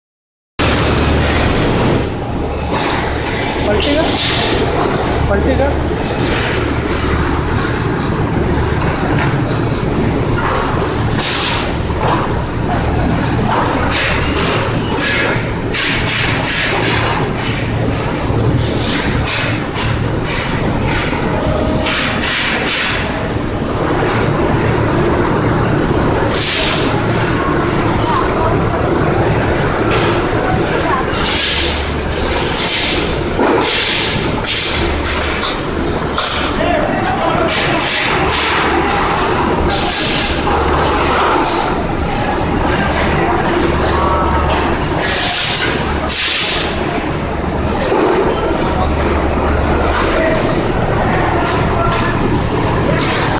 Rusk/ Toast Stacker complete Unit By Soni Engineering works Mumbai - Sumit Yadav 9730228837
Rusk Stacker Unit Design to reduce the labour Dependability & Increase the effeciency of the production in less time for Stacker please contact Sumit yadav - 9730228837 , email our company name Soni Engineering works.